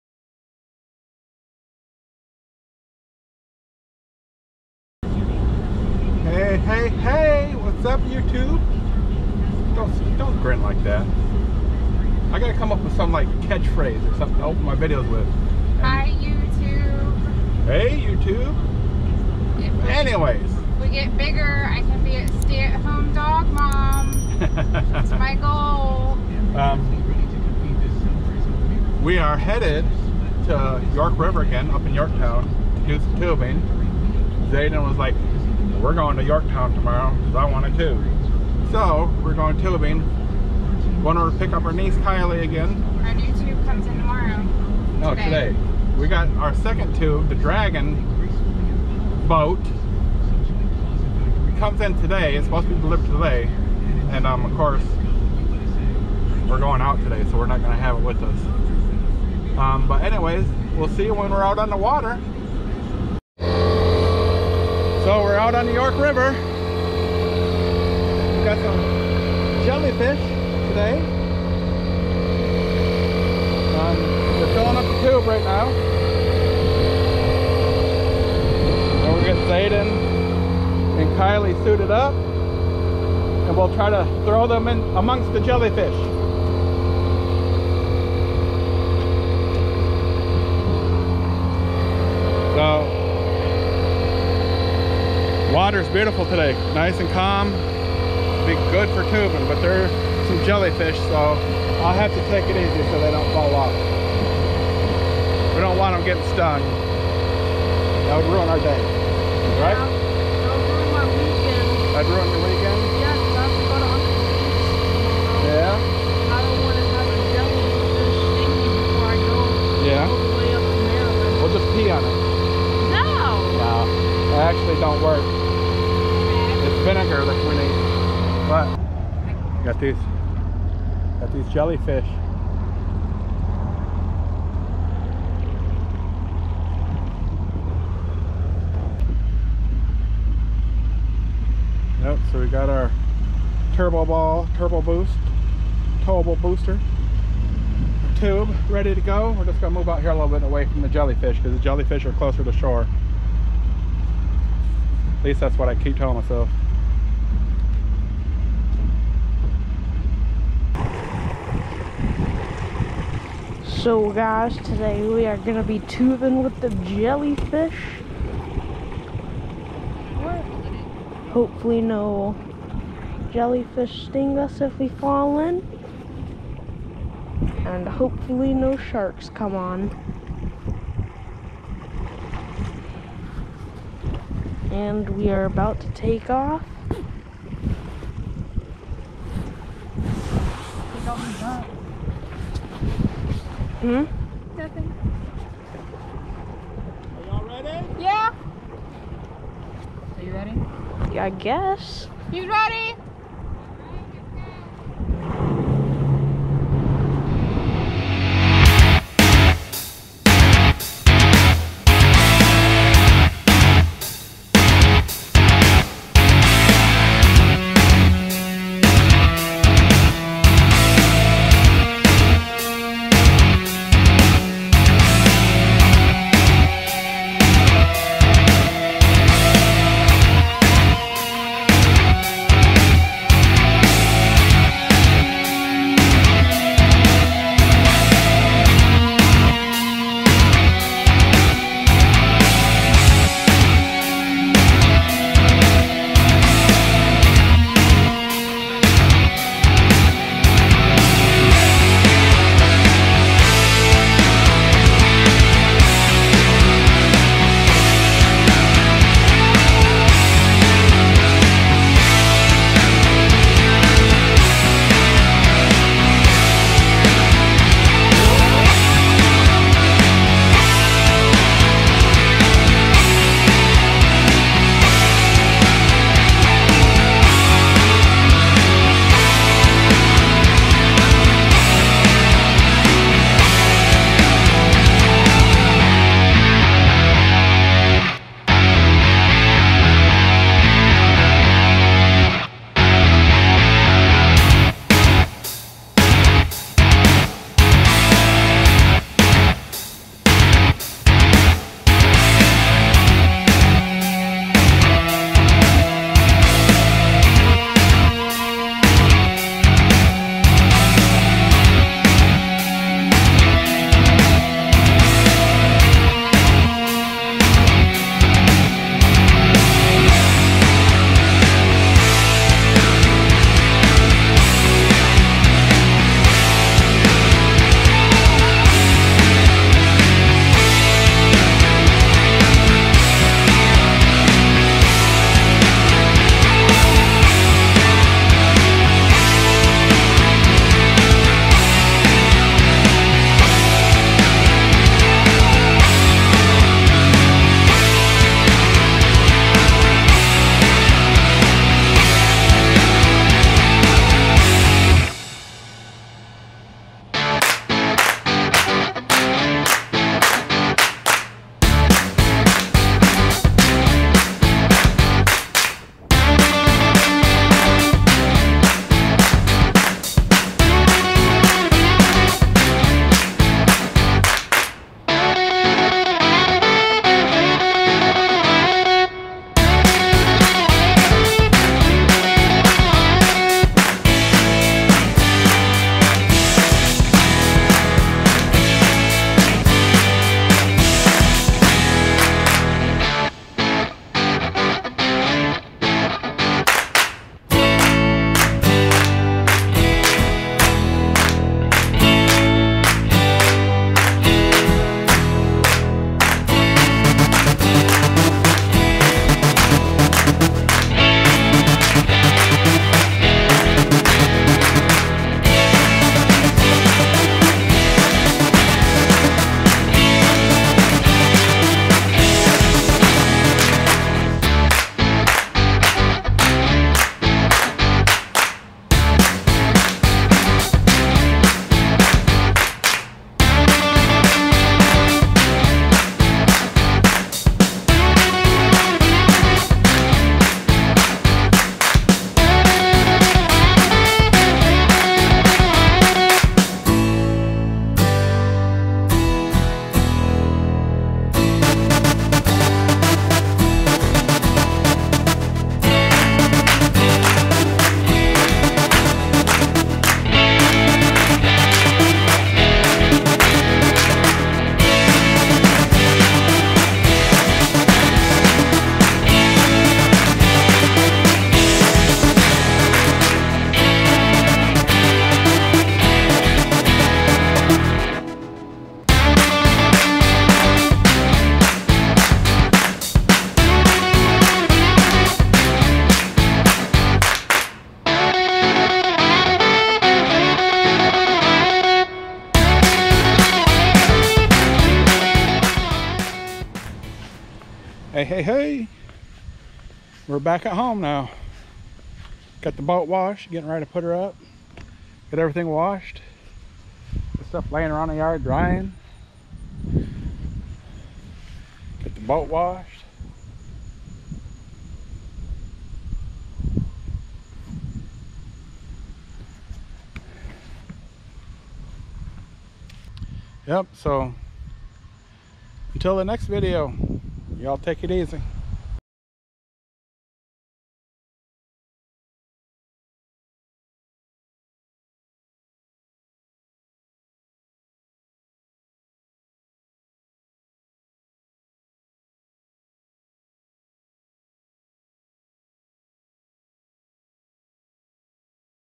Tubing the York River with Jellyfish
Headed to the York River in Yorktown again to do some tubing. Jellyfish were out in force, so I took it easy not to throw the kids into the water.  They still had a blast.